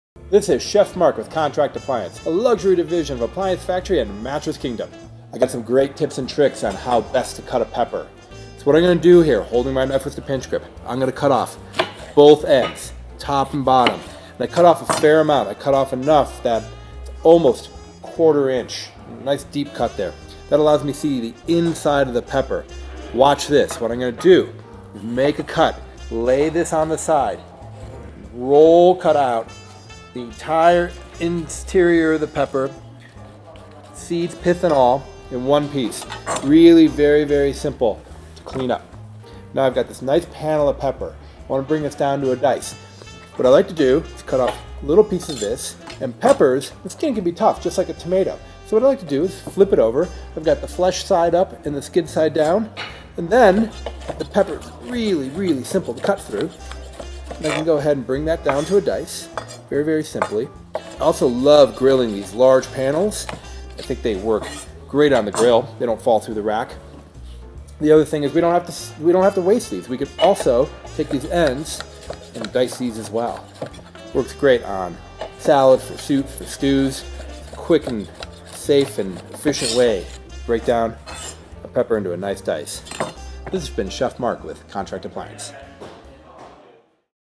Roll cut pepper
Roll cut your pepper.  It's quick and there's no mess